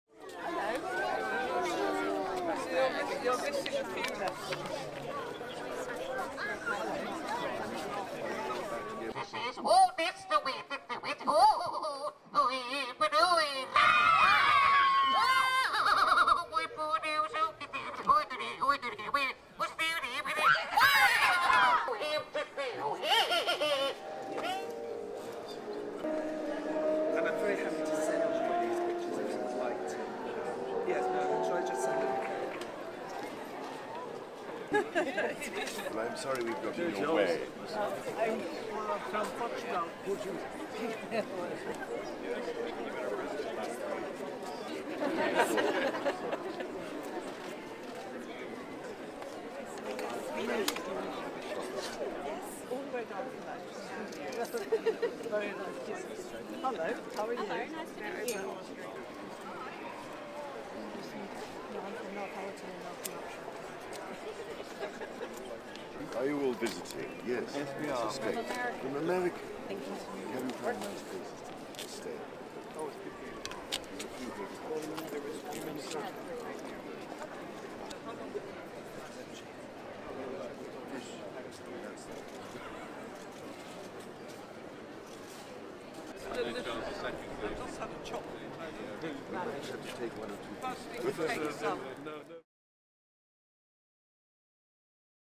King Charles and Queen Camilla in first royal outing since coronation
The newly crowned king and queen performed their first royal engagement since the coronation by visiting a church and market in London's Covent Garden.